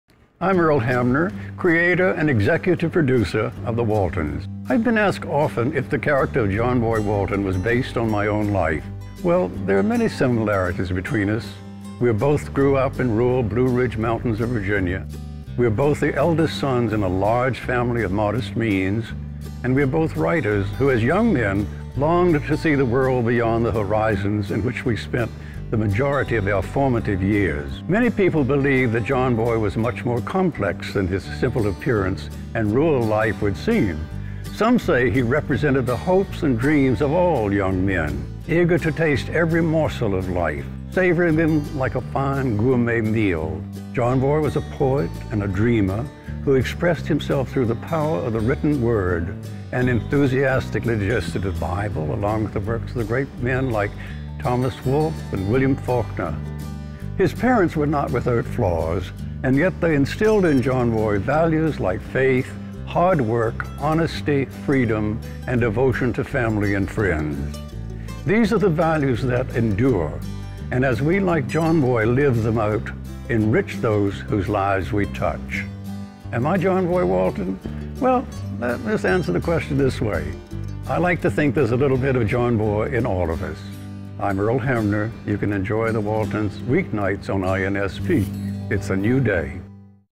INSP presents The Waltons "Earl Hamner - Am I John-Boy?"
Earl Hamner sits down with us to answer the question, am I John-Boy in real life!

WATCH The Waltons at 1PM, 8PM and 9PM ET on INSP!